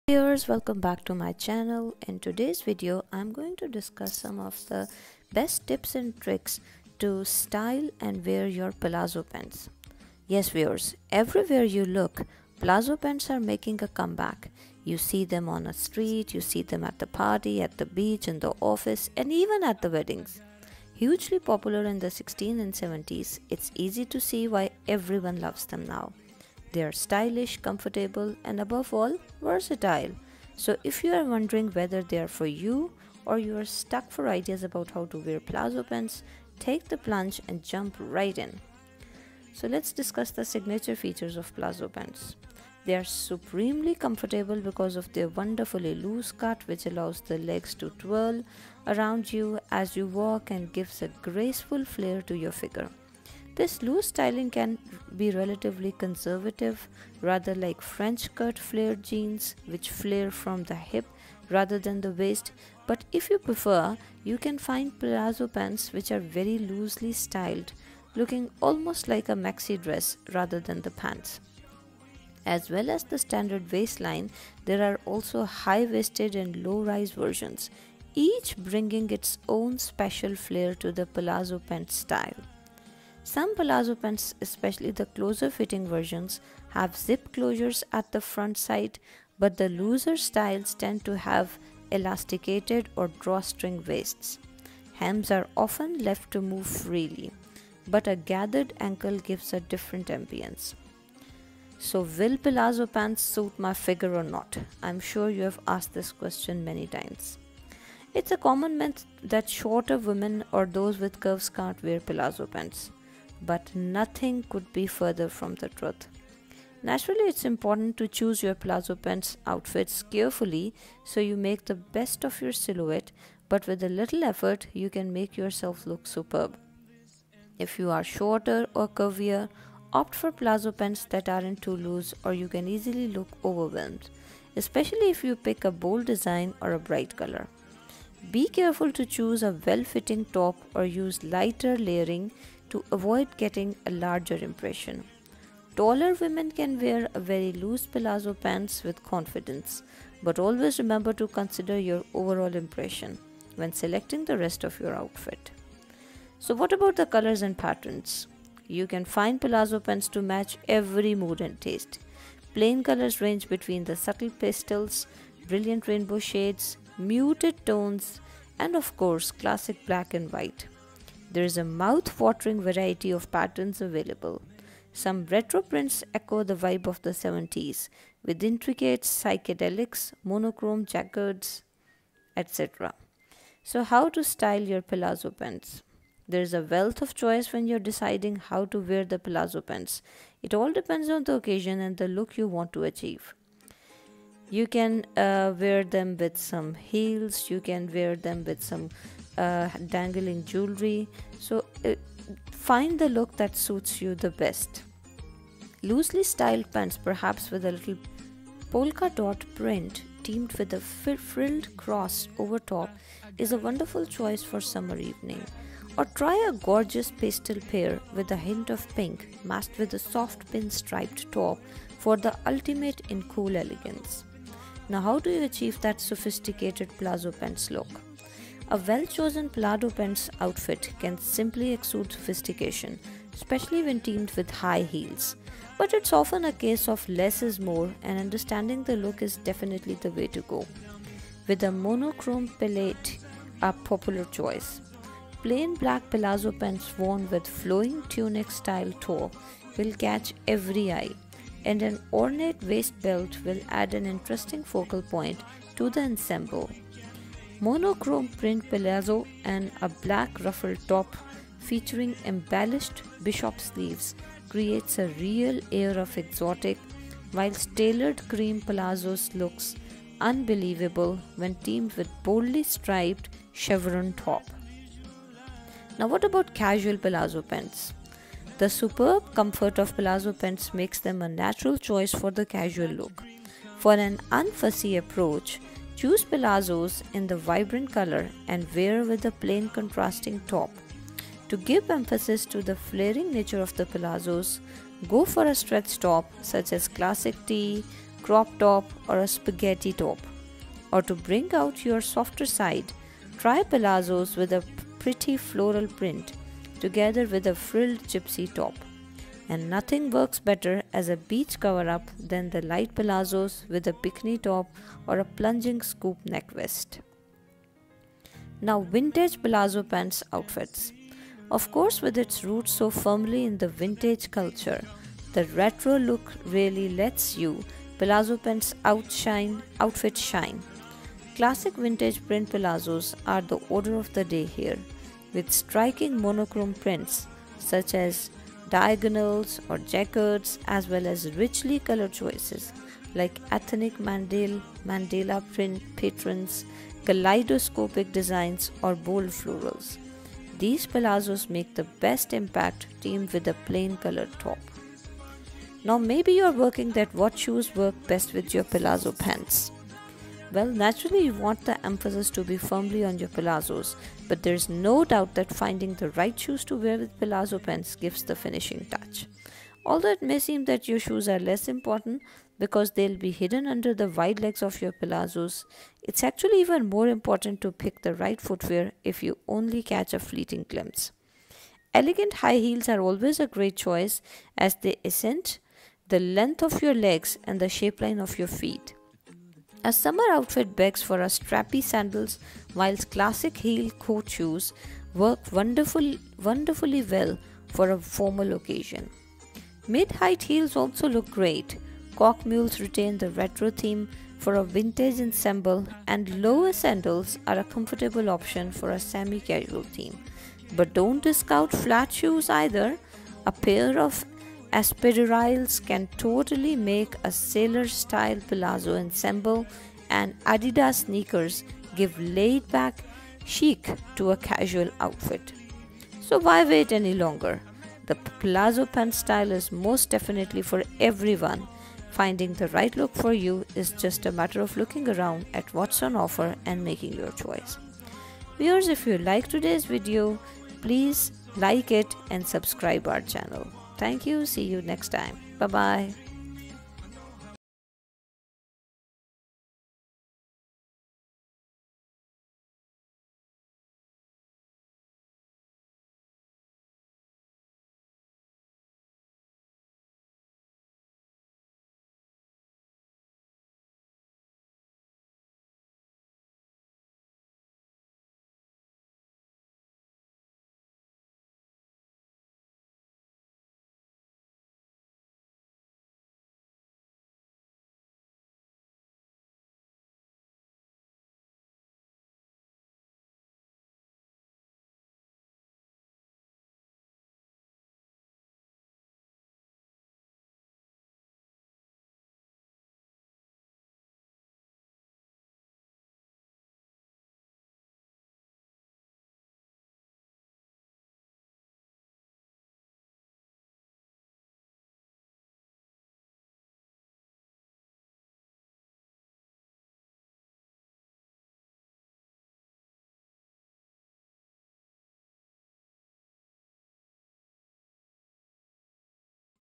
How to style plazzo pants
This video discusses some of the coolest ways you can style your palazzo trousers.